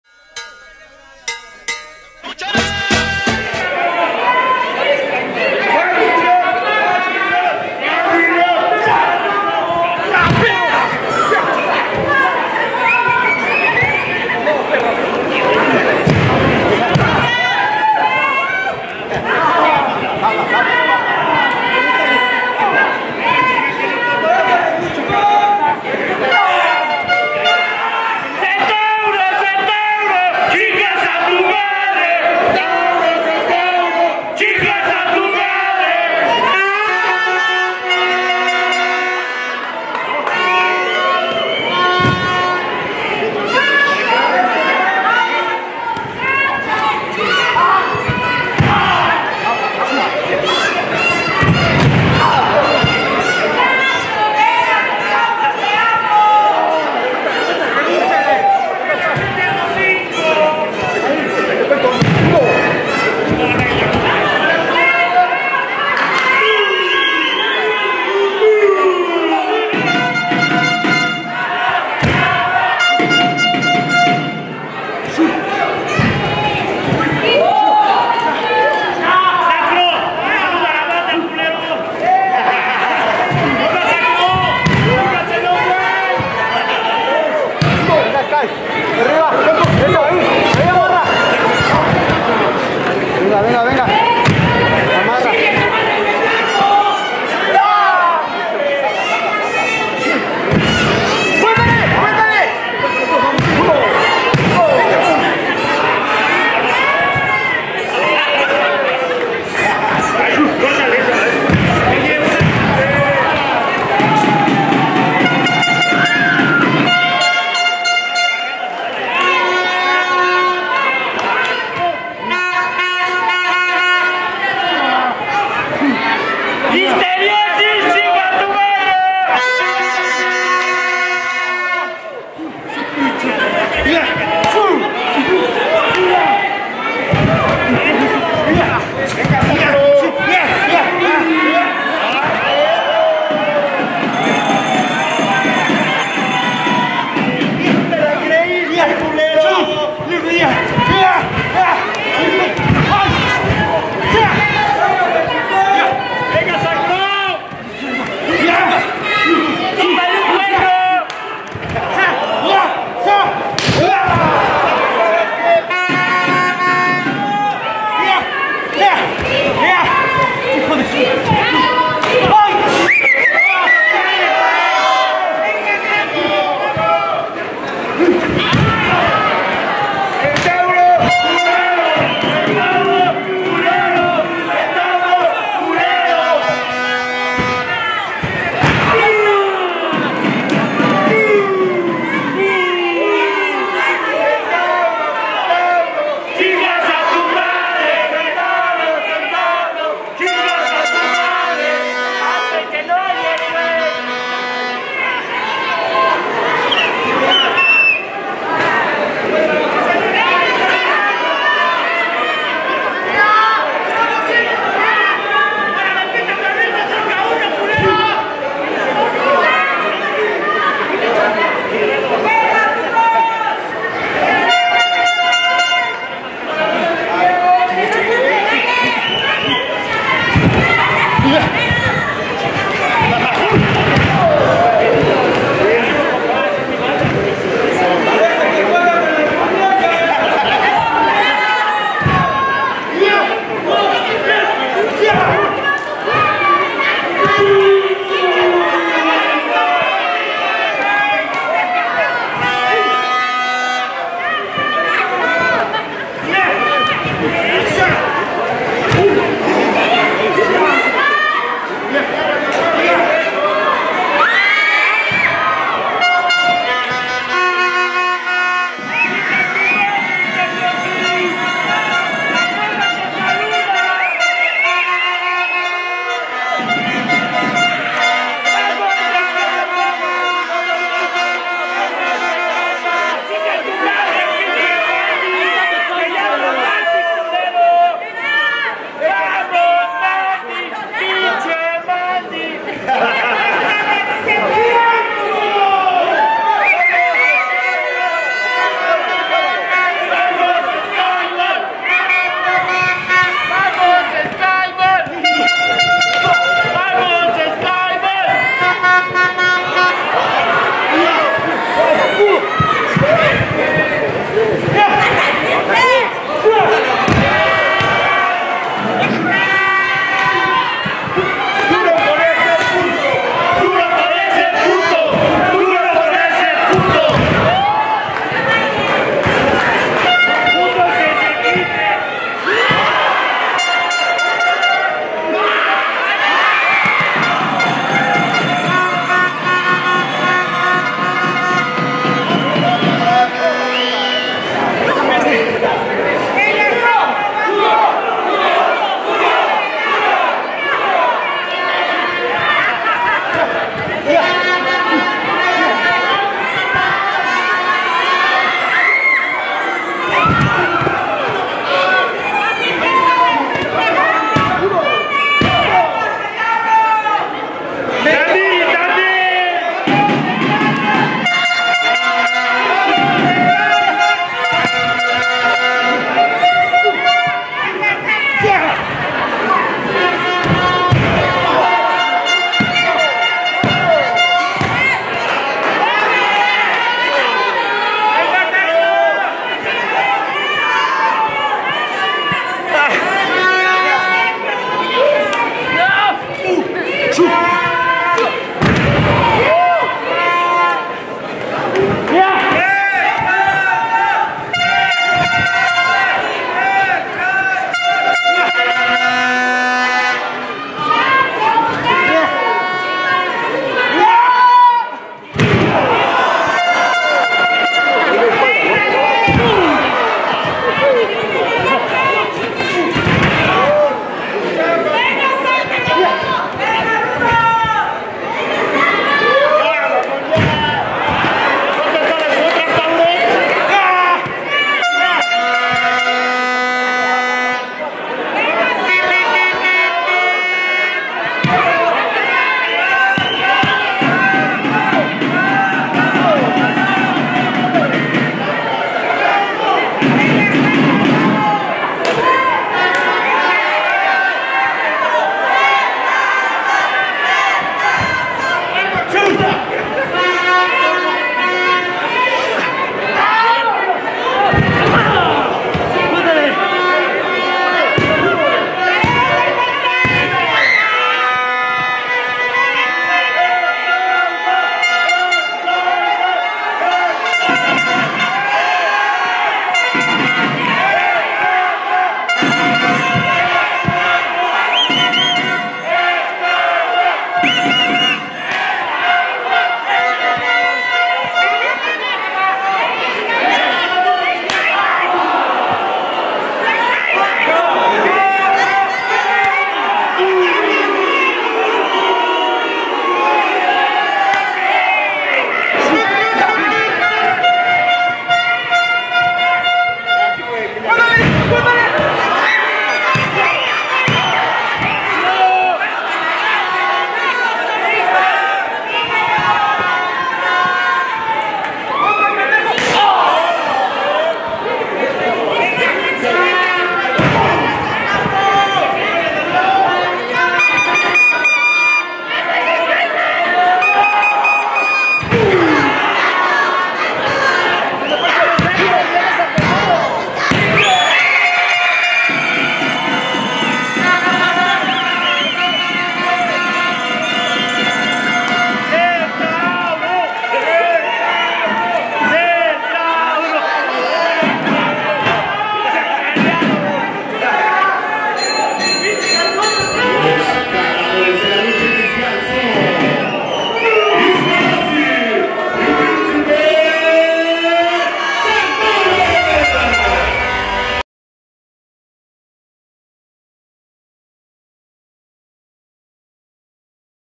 Lucha inicial Sacro y Skyman Vs Príncipe Centauro e Hysteriosis con IWRG en la Arena Naucalpan
Lucha inicial Sacro y Skyman Vs PrincipeCentauro e Hysteriosis con IWRG en la ArenaNaucalpan...